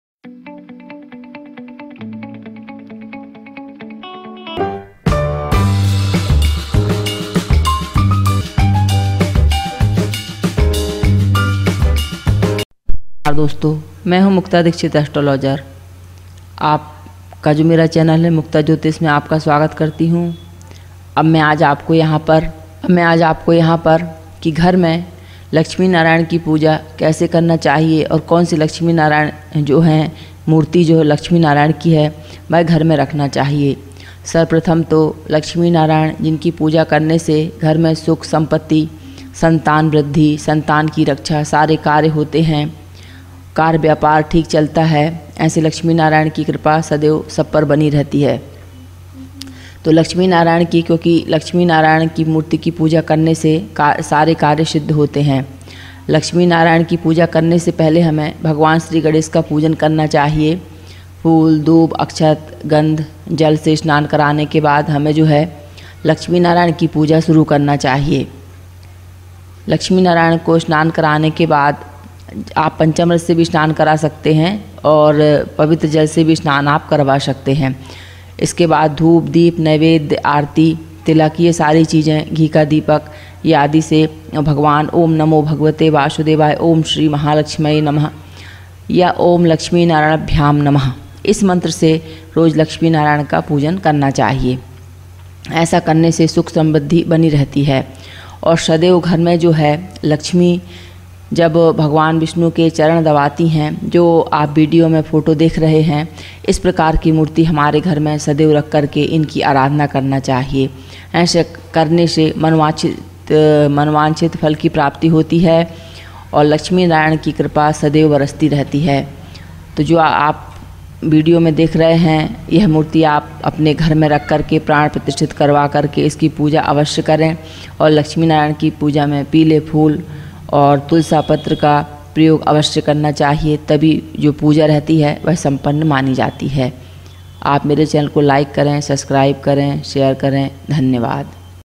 laxmi narayan pujan vidhi लक्ष्मी नारायण की कोन सी मूर्ति पूजा# घर by muktajyotishs
लक्ष्मी नारायण की कोन सी
मूर्ति पूजा घर मे रखनी चाहिए? क्या अर्पित करे
पूजा विधि --
muktajyotishs